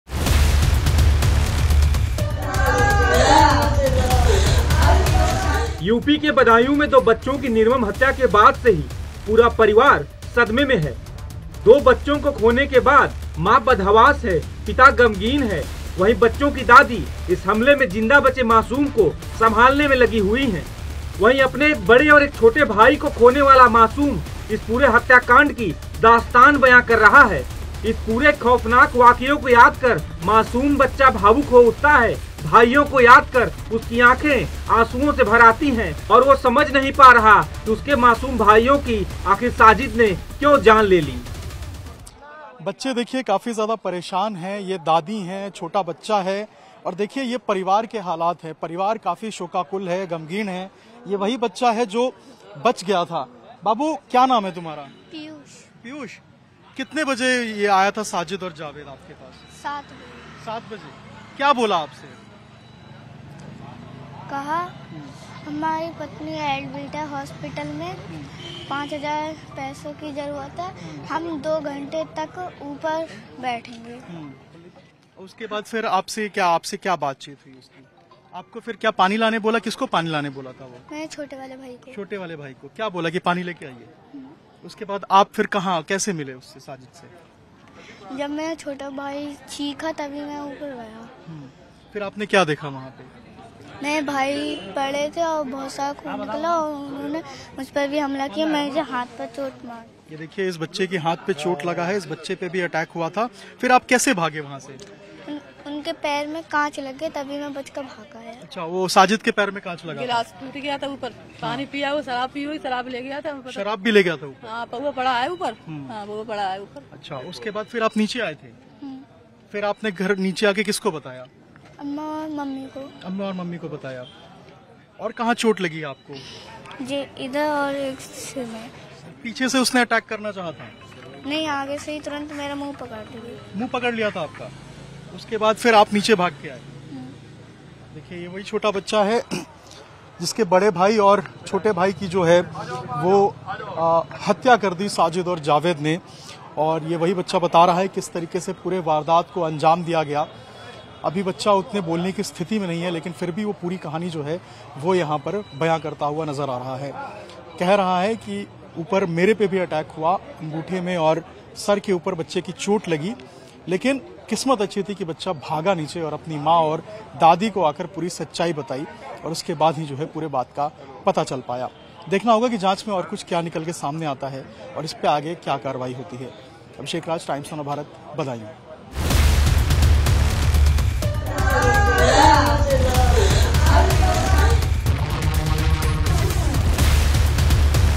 Badaun Case Latest News Update : भाइयों के हत्या की दास्तान मासूम बच्चे ने सुनाई ! | Hindi News
Uttar Pradesh के Badaun में दो बच्चों की निर्मम हत्या के बाद से ही पूरा परिवार सदमे में है। वहीं अपने एक बड़े और एक छोटे भाई को खोने वाला मासूम इस पूरे हत्याकांड की दास्तान बयां कर रहा है। इस पूरे खौफनाक वाकये को याद कर मासूम बच्चा भावुक हो उठता है। इस हत्याकांड में शामिल एक आरोपी Sajid को Police ने Encounter में ढेर कर दिया है।